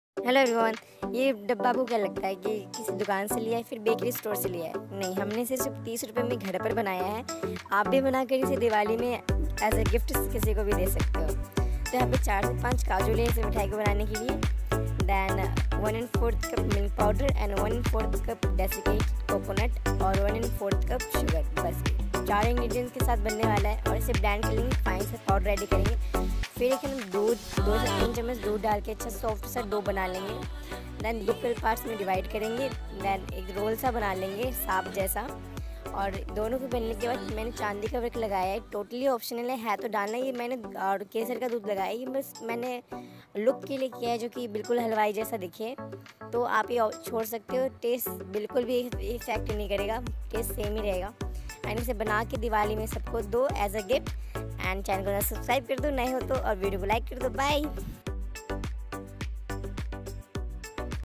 5 काजू और 30₹ में Kaju Roll बनाए। Halwai Style Kaju Roll in 10 minutes‌‌ काजू रोल‌ रेसिपी।
Dosto aaj banayenge ekdum swadisht aur moulth melting bilkul halwai jaisi kaju pista roll mithai aur woh bhi ghar par available ingredients mein. Bohot hi aasan aur simple si recipe hai aap isse ek baar jarur try karein ❤️

If you like my video please share your views in the comment section below

Please
to my YouTube channel cookingart